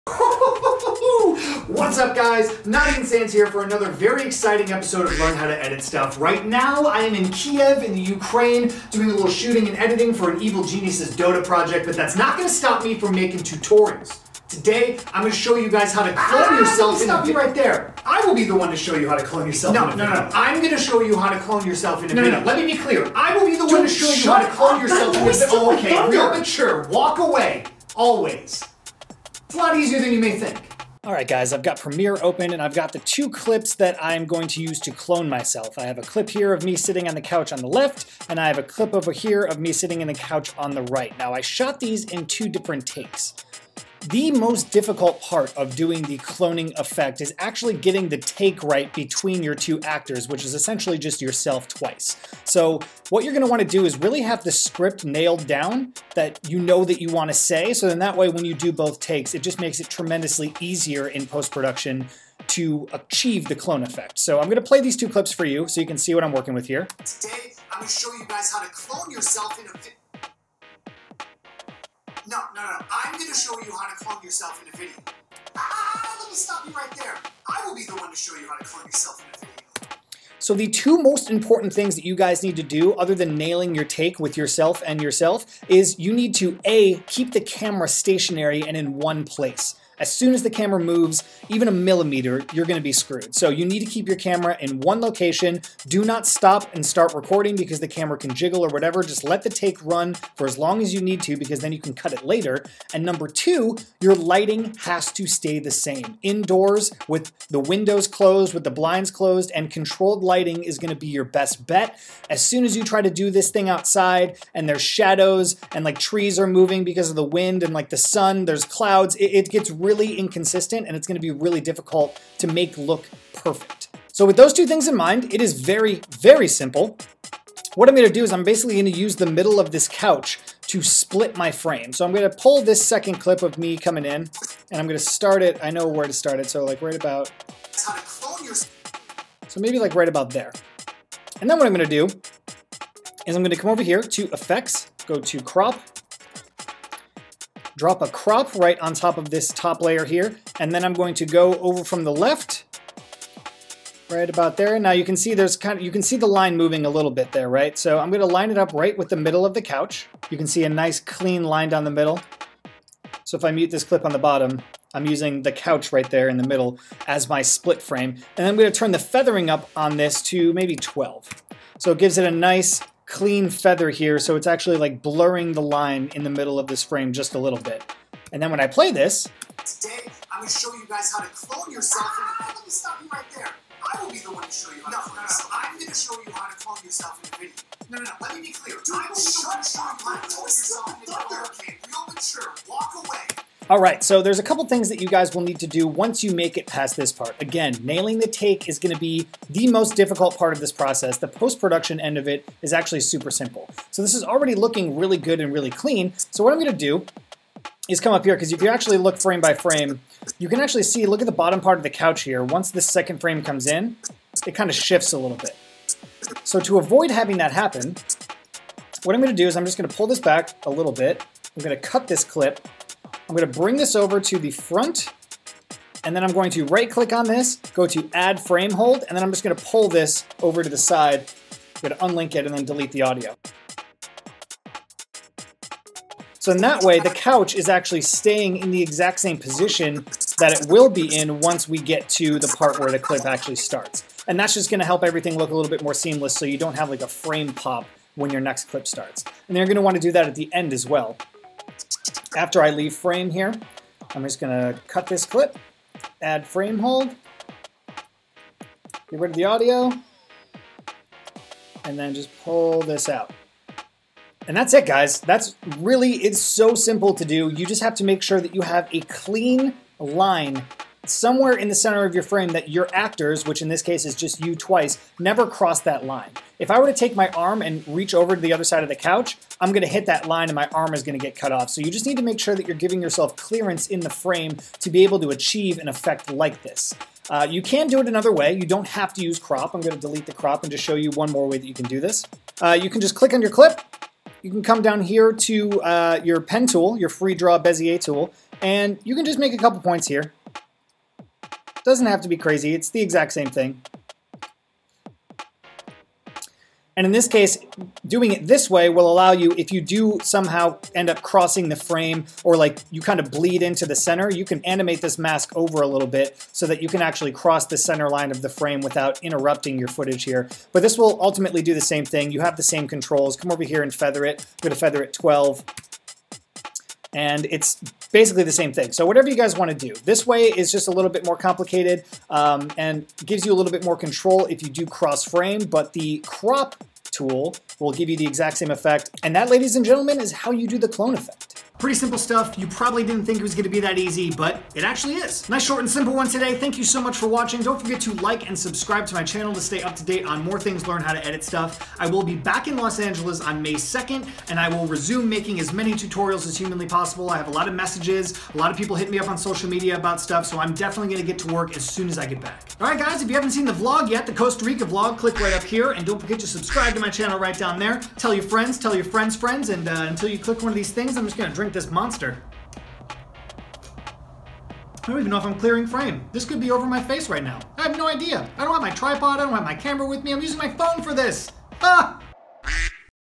How To Clone Yourself In Premiere Pro CC
In this video, learn how to easily clone yourself in a video using Premiere Pro CC! Greetings from the Ukraine